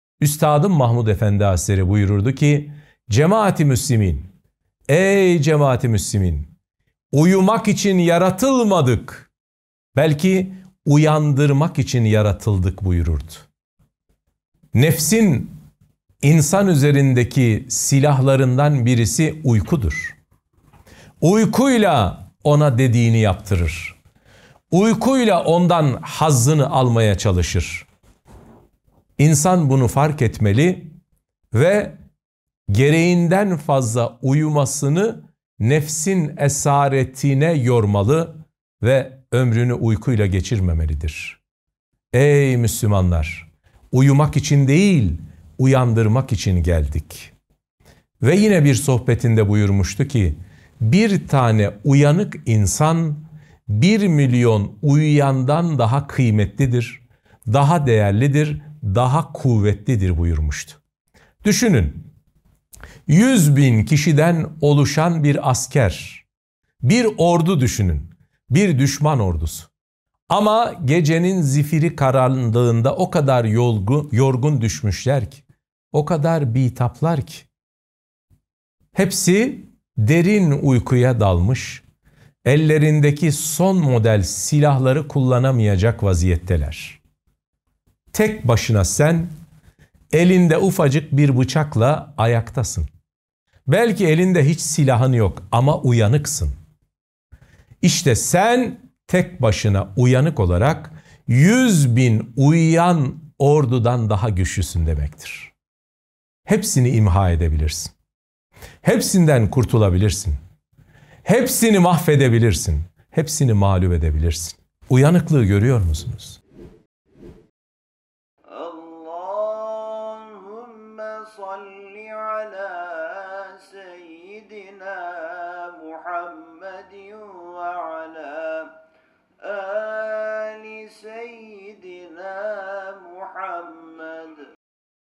Mahmud Efendi Hazretlerimiz "Uyumak İçin Yaratılmadık!" Buyurdular - Mahmud Eren Hoca Efendi
Mahmud Eren Hoca Efendi, fazla uyumanın nefsin oyunlarundan bir oyunu olduğunu ve uyku saatlerimize dikkat etmemiz gerektiğini anlatmaktadır.

İsmailağa NET tarafından hazırlanan video kesitlerini paylaşarak sohbetlerin daha geniş kitlelere ulaşmasına vesile olabilirsiniz.